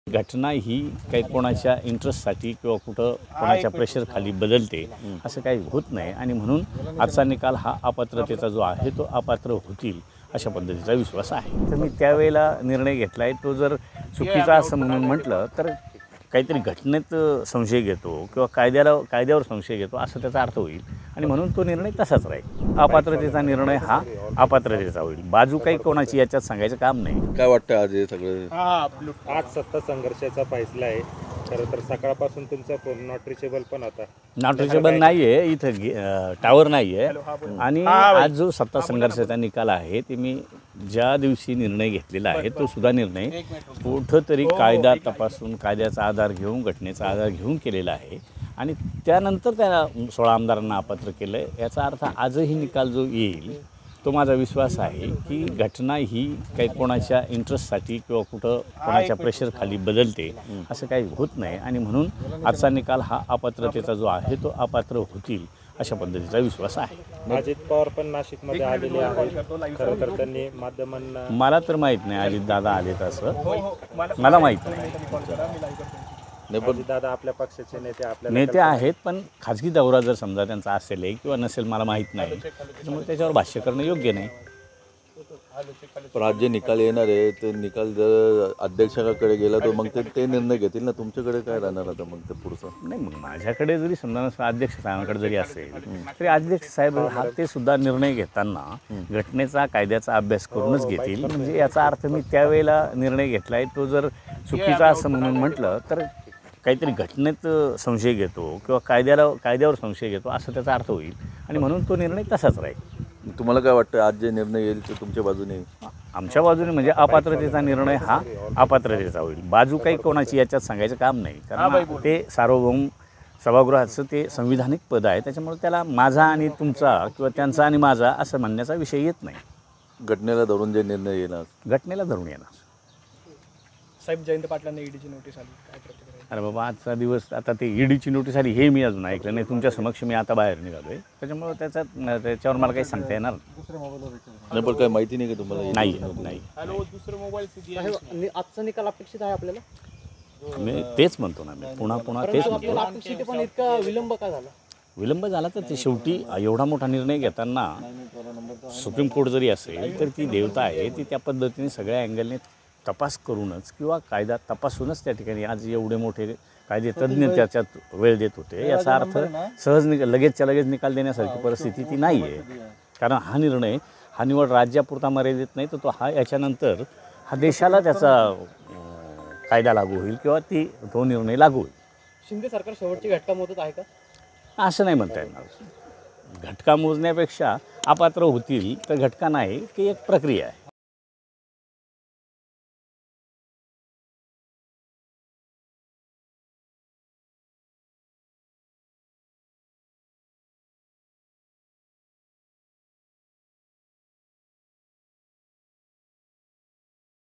Narhari Zirwal | ...तर हे प्रकरण विधानसभा अध्यक्षांकडेच पाठवलं जाईल | LetsUpp Marathi
सर्वोच्च न्यायालयात राज्यातील सत्ता संघर्षाची सुनावणी सुरु आहे. त्यापूर्वी वकील असीम सरोदे यांनी मोठं भाष्य केलं आहे.